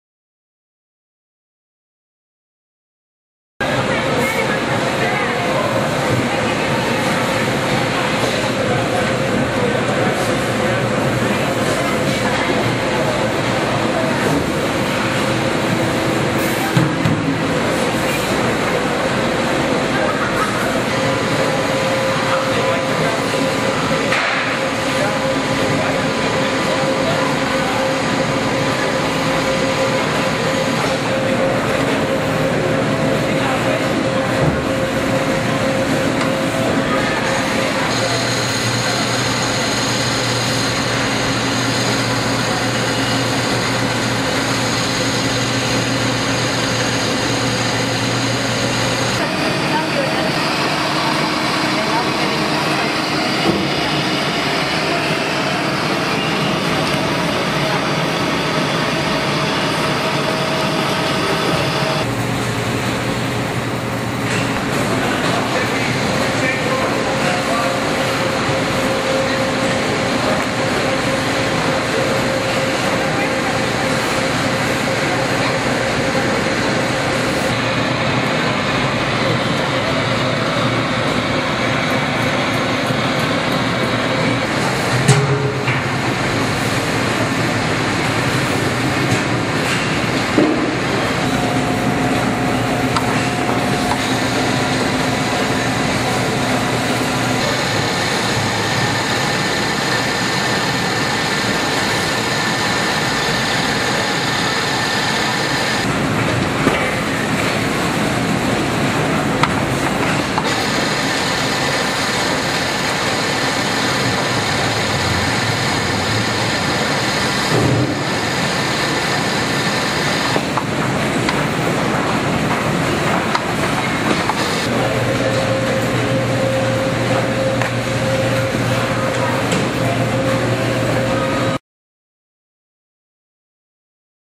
Vemag LPG208 Sausage Linker & TM203 Cutter
Vemag Sausage Stuffer with LPG208 Length Portioning Machine with Twin Horns and TM203 Link Cutter from Reiser. The LPG208 is a high-speed length portioning machine that features two rotating linking horns to make it the fastest linker in the industry. The TM203 cuts strings of linked sausage into individual sausages quickly and precisely.